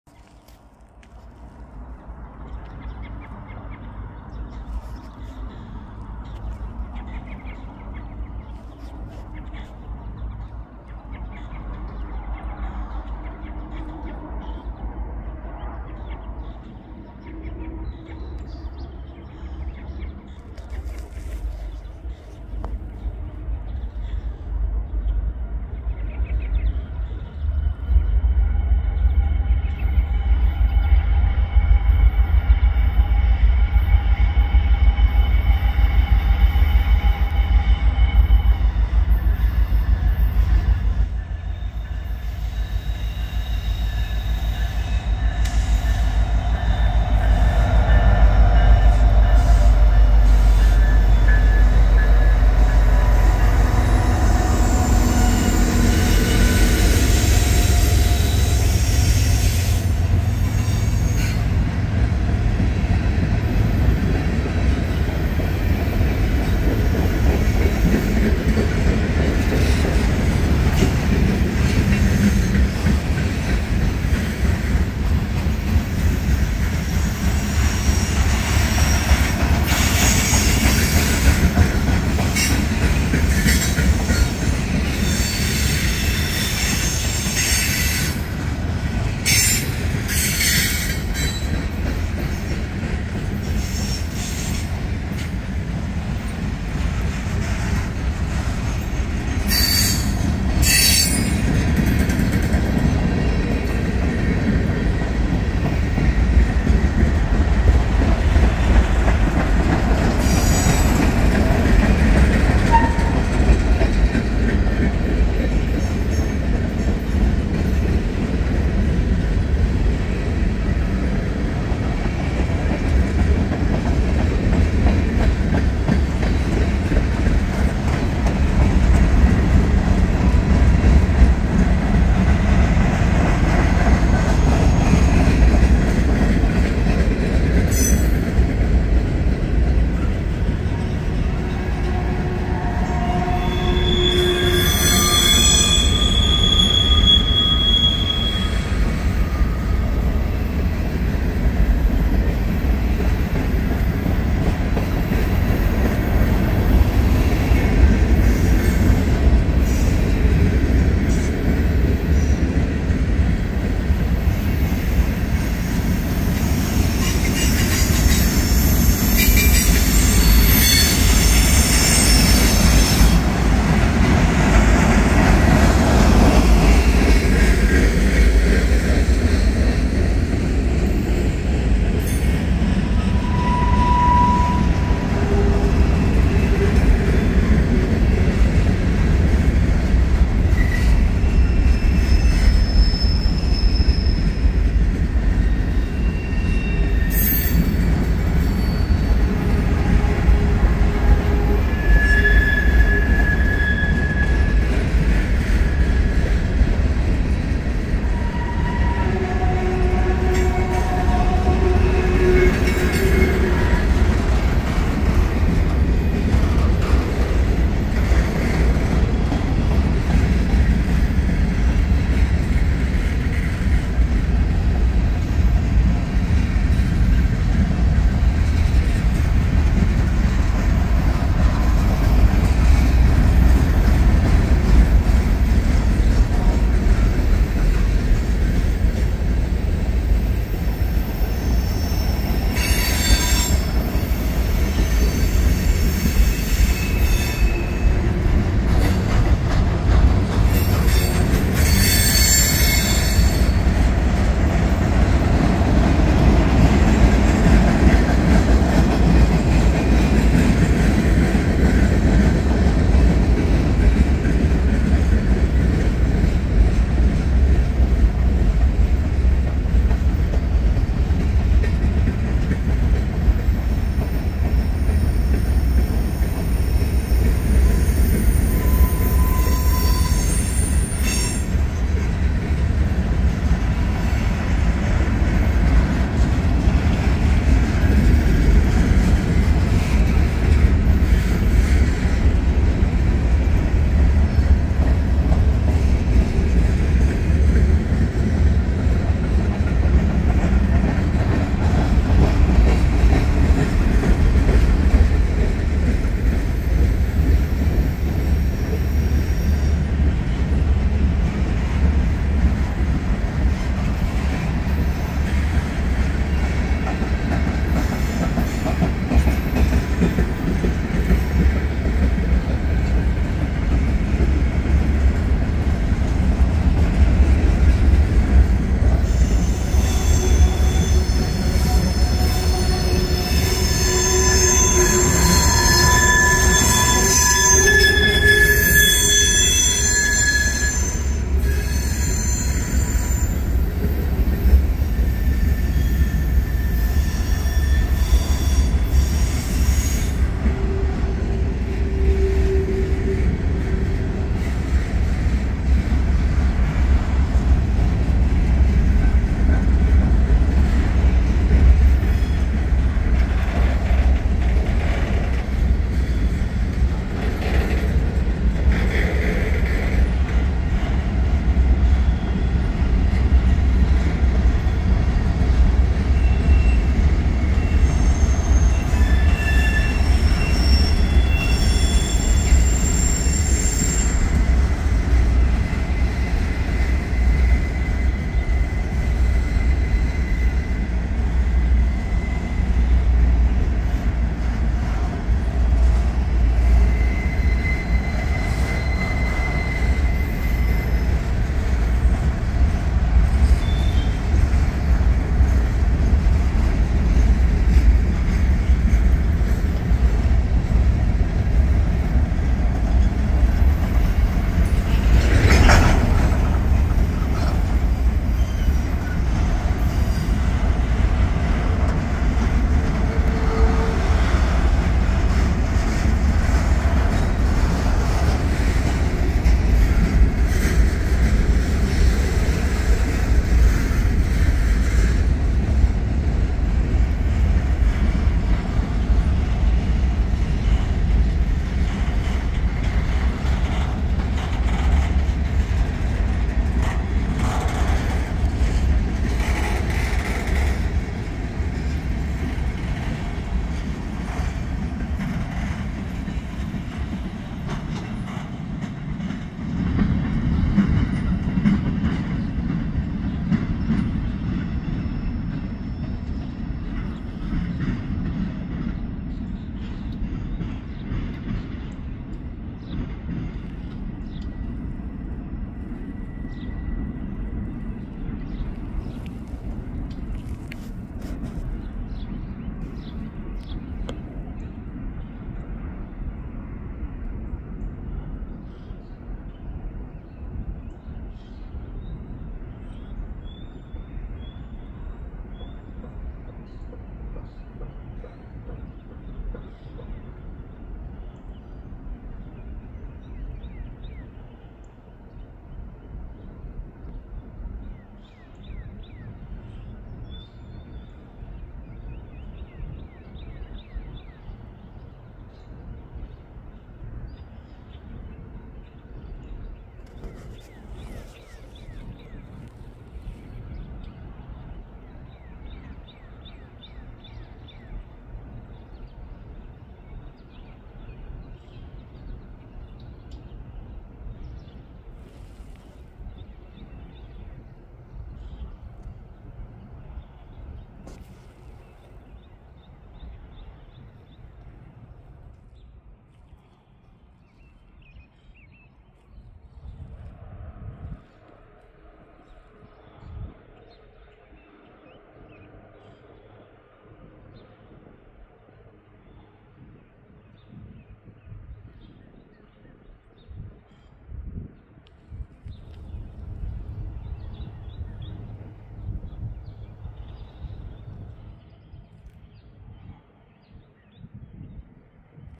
Norfolk Southern #1843 leads a 145 car manifest at Lewistown, PA (04/25/2024)
11V